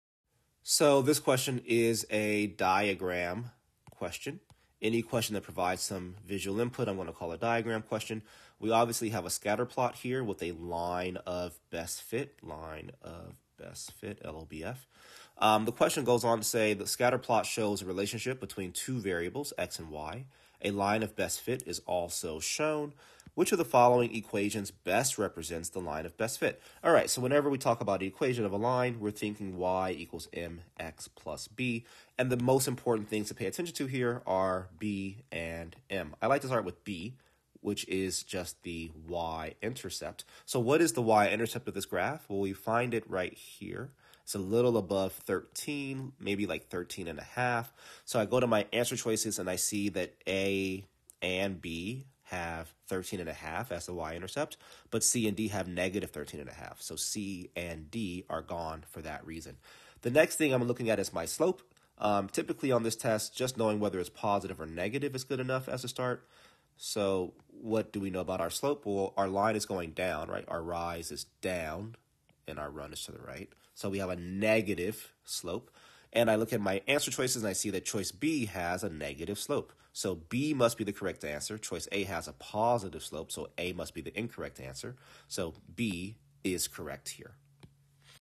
The scatterplot shows the relationship between two variables, x and y. A line of best fit is also...
Nonadaptive Digital SAT Practice Test 3 Module 2 Question 11:

The scatterplot shows the relationship between two variables, x and y. A line of best fit is also shown.  Which of the following equations best represents the line of best fit shown?